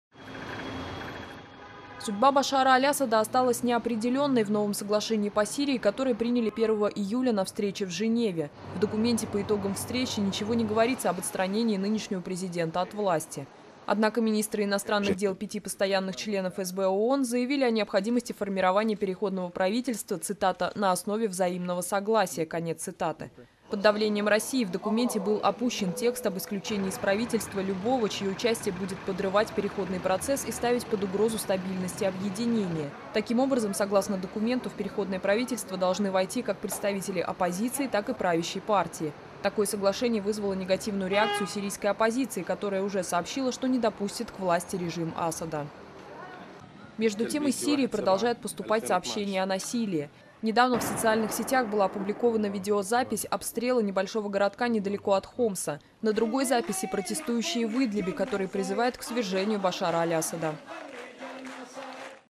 Оппозиция недовольна соглашением по Сирии
) В Женеве прошла встреча постоянных членов ООН по Сирии. Они приняли соглашение, согласно которому власть в Сирии должна быть разделена между оппозицией и правящим режимом.